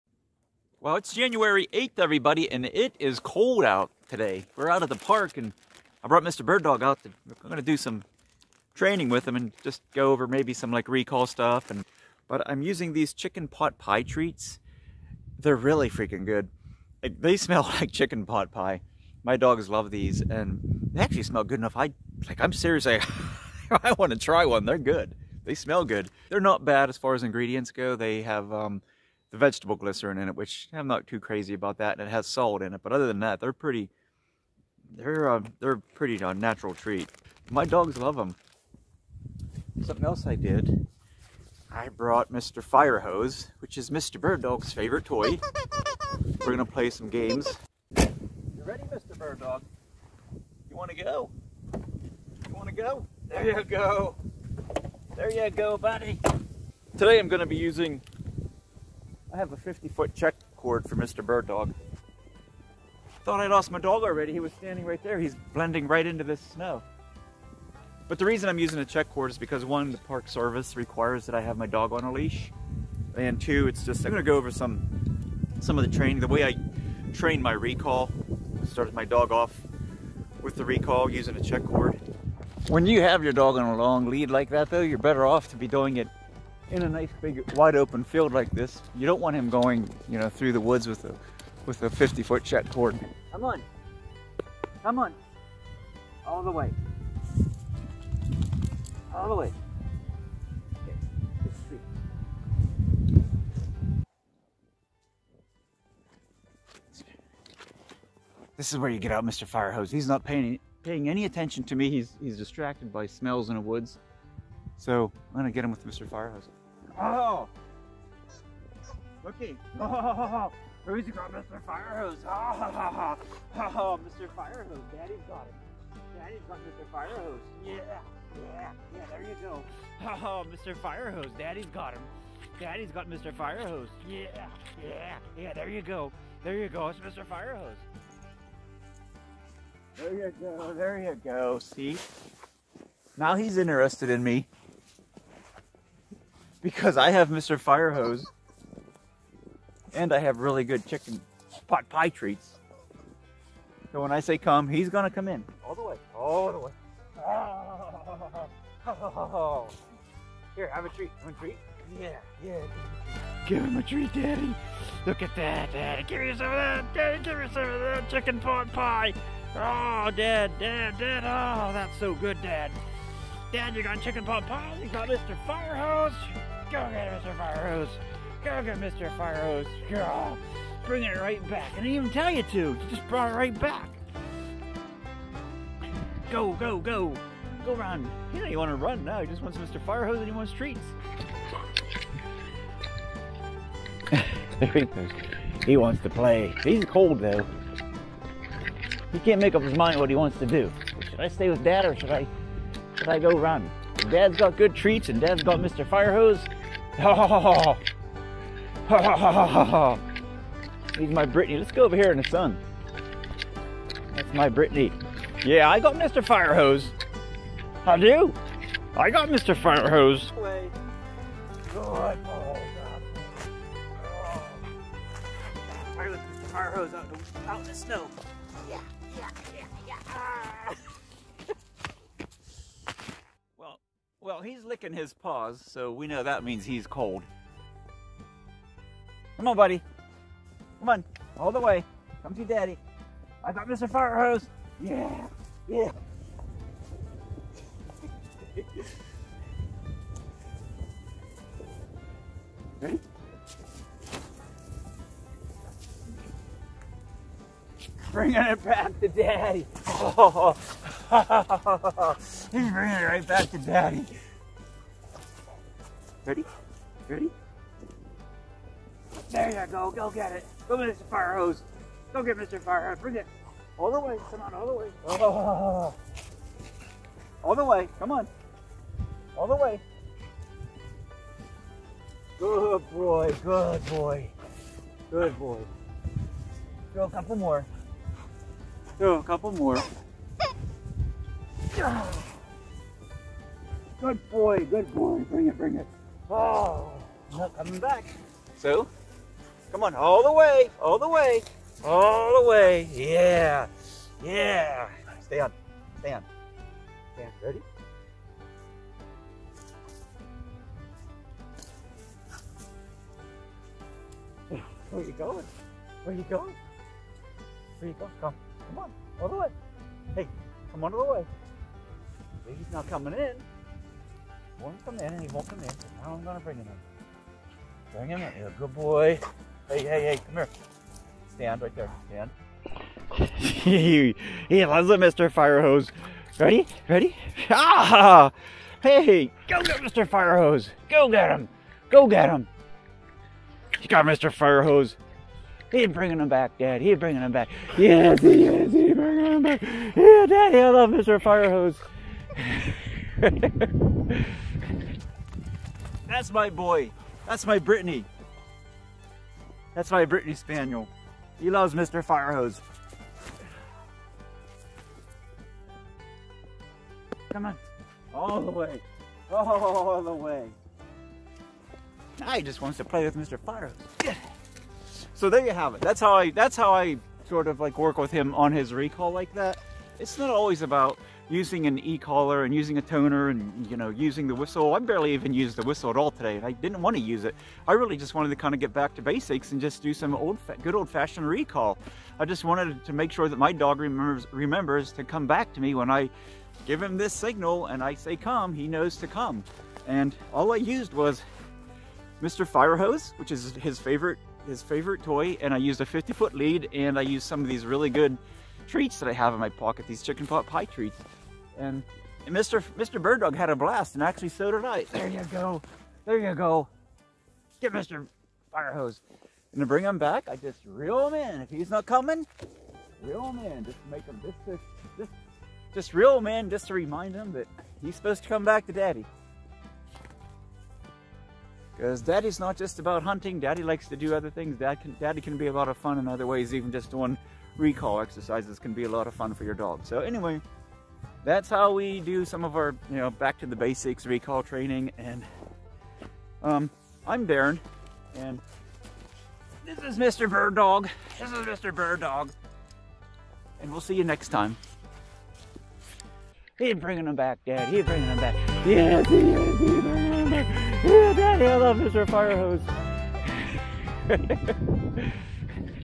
Bonding with My Brittany Dog and Revisiting Basic Recall Commands
An afternoon of bonding time and basic recall practice with my Brittany Spaniel dog using his favorite toy and some irresistible chicken pot pie dog treats on this cold January day in 2022. Mr. Birddog had a blast on this day and so did I.

*I'm learning to use DaVinci Resolve as my video editor so just ignore the bad callout attempts that you might notice in the beginning of this video.